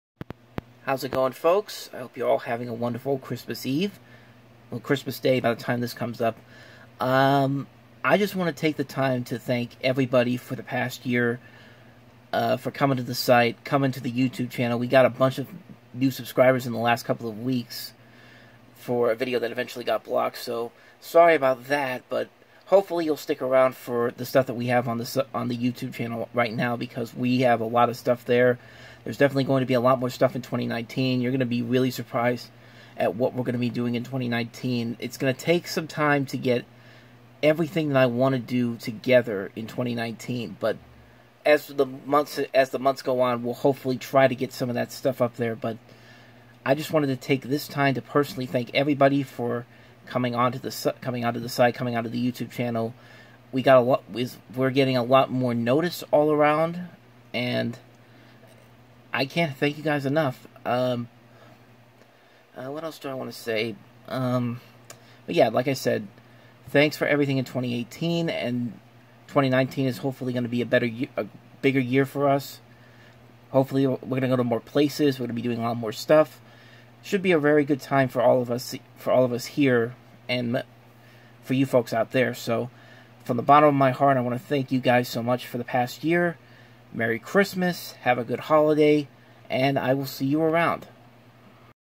Merry Christmas From The Reviewing Network
Just a little holiday greeting from everyone who has helped to get more exposure in 2018 and hopefully 2019 will be bigger and better.

From all of us, Merry Christmas from The Reviewing Network.